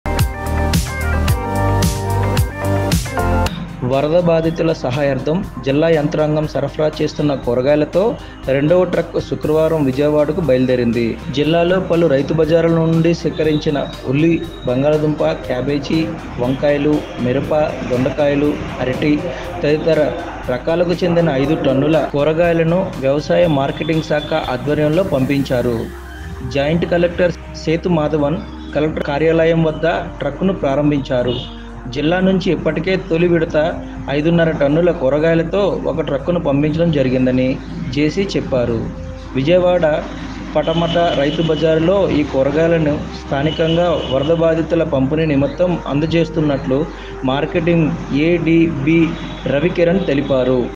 విజయవాడ వరద బాధితుల సహాయార్థం పంపిస్తున్న కూరగాయల వాహనాన్ని ప్రారంభించిన JC ఎస్ సేతు మాధవన్
# వ‌ర‌ద బాధితుల కోసం రెండో విడ‌త కూర‌గాయ‌లు స‌ర‌ఫ‌రా

# ఐదు ట‌న్నుల‌తో విజ‌య‌వాడ‌కు మ‌రో ట్ర‌క్కు

# ప్రారంభించిన జాయింట్ కలెక్ట‌ర్ సేతు మాధ‌వ‌న్‌

విజ‌య‌న‌గ‌రం, సెప్టెంబ‌రు 13 :

వ‌ర‌ద బాధితుల స‌హాయార్ధం జిల్లా యంత్రాంగం స‌ర‌ఫ‌రా చేస్తున్న కూర‌గాయ‌ల‌తో రెండ‌వ ట్ర‌క్కు శుక్ర‌వారం విజ‌య‌వాడ‌కు బ‌య‌లుదేరింది. జిల్లాలో ప‌లు రైతుబ‌జార్‌ల నుంచి సేక‌రించిన ఉల్లి, బంగాళాదుంప‌, క్యాబేజ్‌, వంకాయ‌లు, మిర‌ప‌, దొండ‌కాయ‌లు, అర‌టి త‌దిత‌ర ర‌కాల‌కు చెందిన ఐదు ట‌న్నుల‌ కూరగాయ‌ల‌ను వ్య‌వ‌సాయ మార్కెటింగ్ శాఖ ఆధ్వ‌ర్యంలో పంపించారు. జాయింట్ క‌లెక్ట‌ర్ ఎస్‌.సేతు మాధ‌వ‌న్ క‌లెక్ట‌ర్ కార్యాల‌యం వ‌ద్ద ట్ర‌క్కును ప్రారంభించారు. జిల్లా నుంచి ఇప్ప‌టికే తొలివిడ‌త ఐదున్నర ట‌న్నుల కూర‌గాయ‌ల‌తో ఒక ట్ర‌క్కును పంపించ‌డం జ‌రిగింద‌ని జె.సి. చెప్పారు. విజ‌య‌వాడ ప‌ట‌మ‌ట రైతుబజార్‌లో ఈ కూర‌గాయ‌ల‌ను స్థానికంగా వ‌ర‌ద బాధితుల పంపిణీ నిమిత్తం అంద‌జేయ‌నున్న‌ట్టు మార్కెటింగ్ ఏ.డి. బి.ర‌వికిర‌ణ్ తెలిపారు.